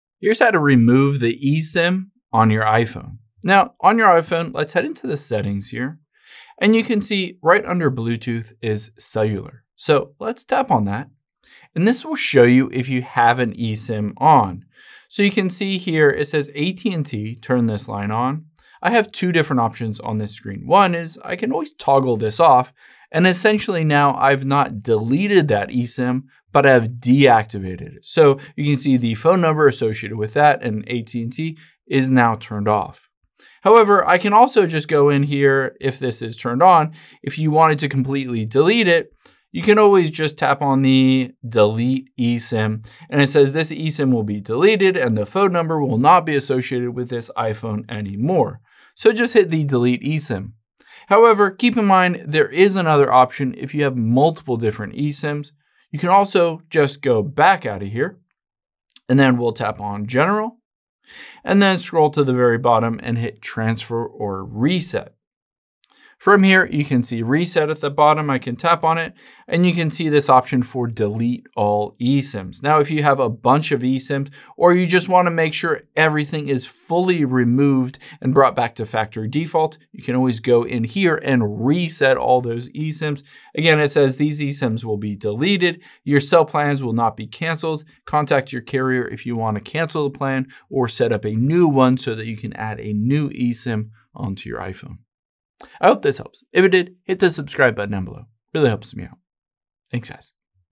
How To Remove eSIM On iPhone
Let's remove any of your old eSIMs on your iPhone if you changed carriers or are getting ready to sell or give away your phone, etc.